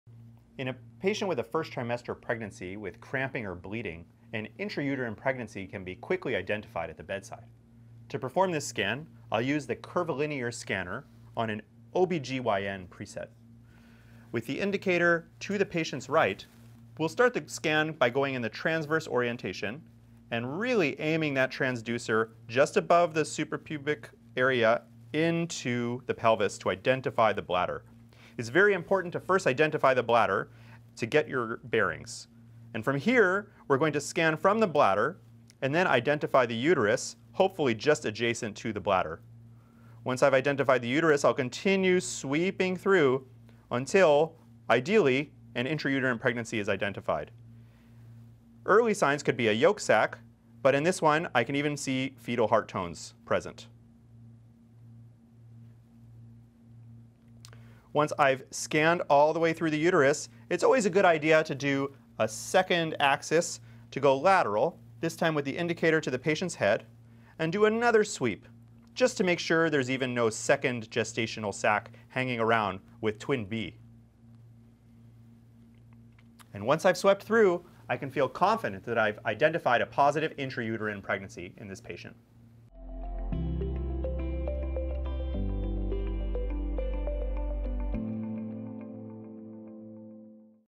First Trimester Intrauterine Pregnancy: Ultrasound Scanning Technique
Always first Identify the bladder by aiming into pelvis from just above pubic symphysis. Then identify  the uterus directly adjacent to bladder. An intrauterine fetal pole can be reliably identified if heart movements are identified or a definitive yolk sac is seen within the uterus.